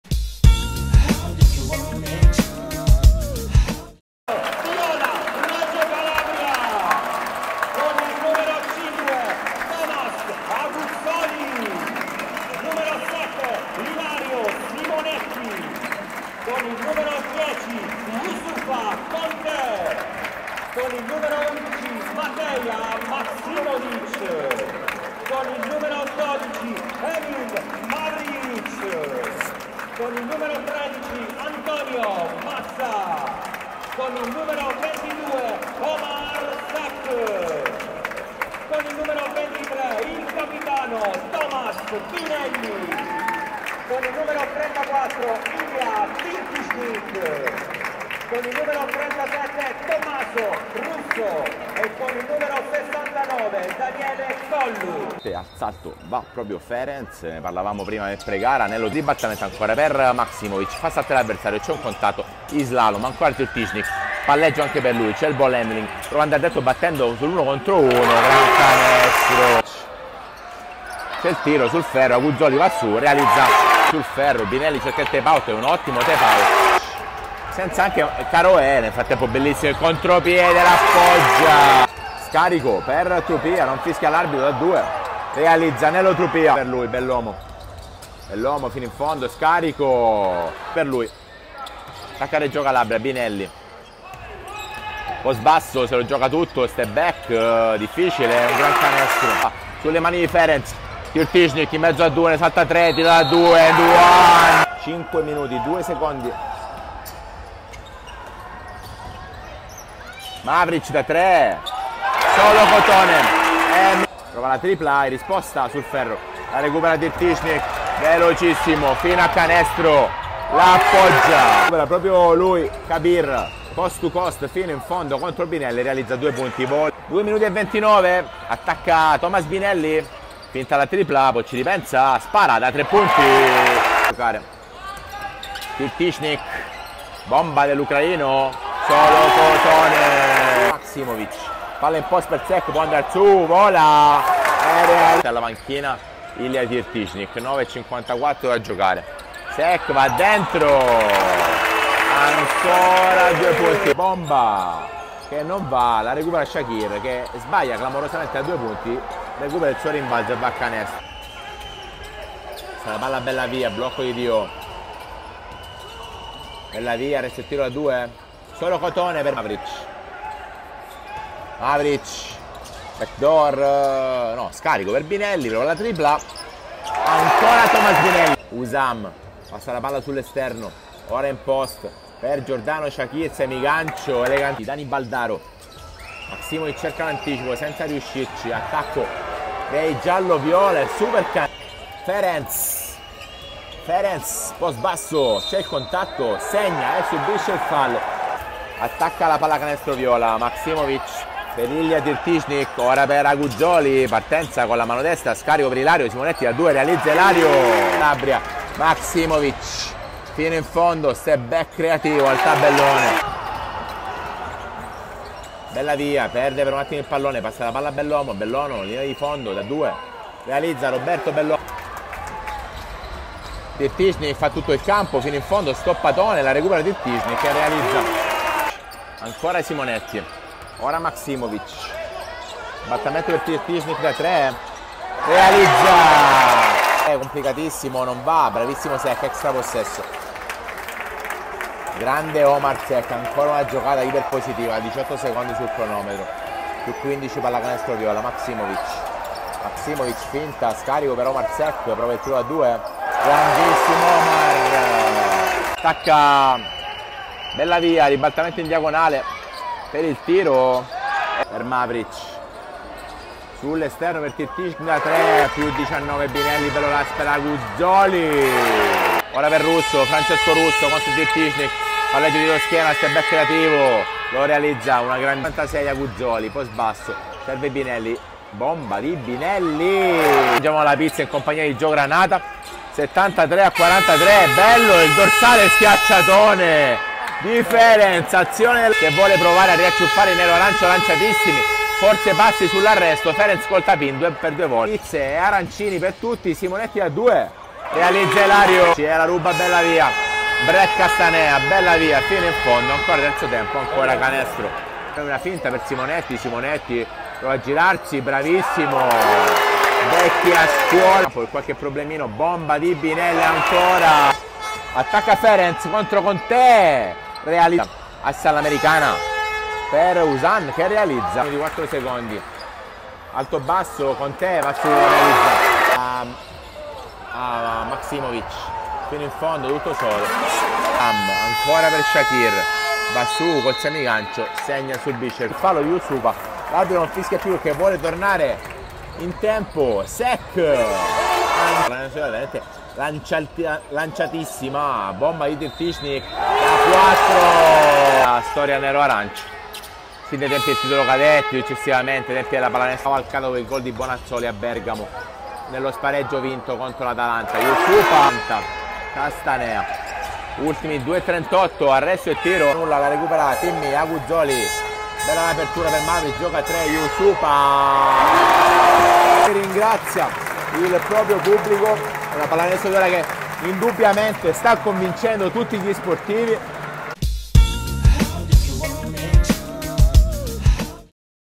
PALLACANESTRO VIOLA-CASTANEA DI MESSINA
Il film della partita del PalaCalafiore: i neroarancio si confermano in vetta.